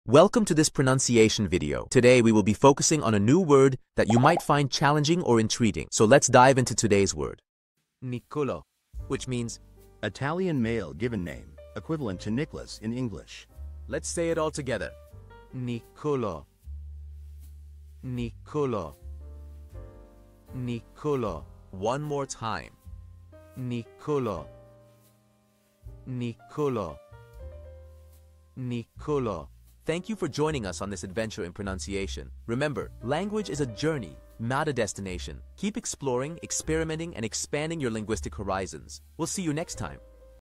How to pronounce Niccolò
Master the Pronunciation of 'Niccolò Which Means Niccolò' - which means : Italian evil Given Name, equivalent to nicholas in English. 📝 with @PizzaPronunciation 🎓🗣️ - [Your Guide to Italian]

Learn to pronounce 'Niccolò' perfectly in Italian with our easy and engaging tutorial by @PizzaPronunciation! 📖📽️ In this comprehensive video, we provide you with native speaker examples 🗣️👂 and phonetic breakdowns 🎵 to ensure you get the pronunciation just right.

How to say Niccolò in Italian